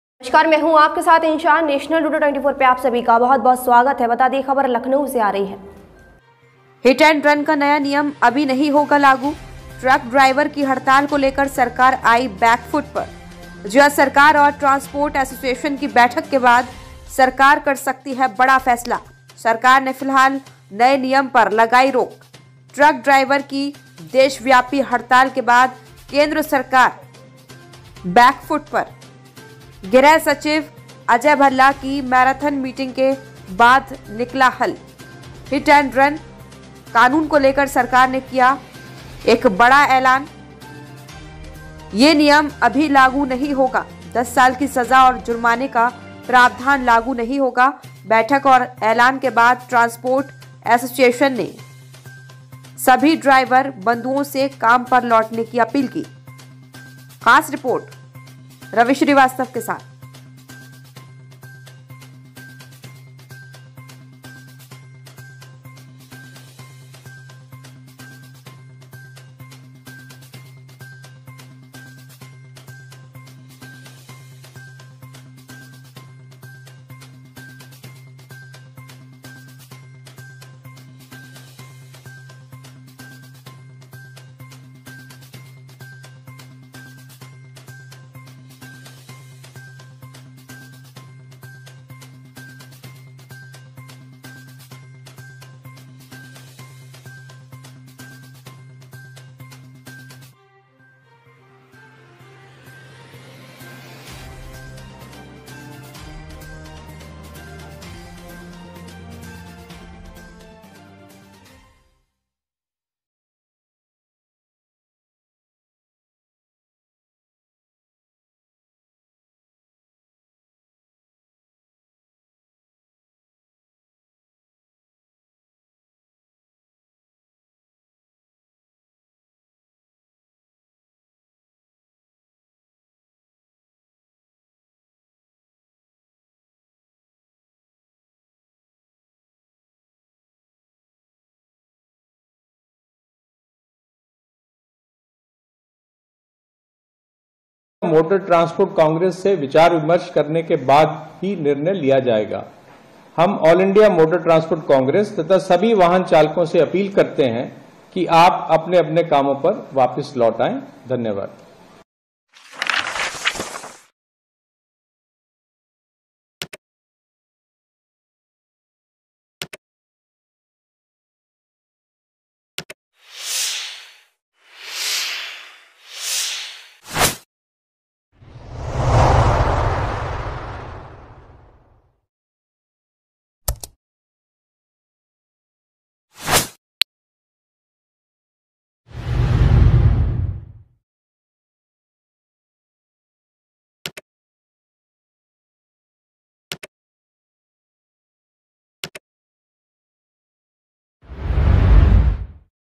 लखनऊ- सूत्रों के हवाले से बड़ी खबर,केंद्र सरकार के नए नियम वाला नया कानून अभी नही होगा लागू।
लखनऊ- सूत्रों के हवाले से बड़ी खबर,केंद्र सरकार के नए नियम वाला नया कानून अभी नही होगा लागू, ट्रक ड्राइवर की हड़ताल को लेकर सरकार जा सकती है बैक फुट पर।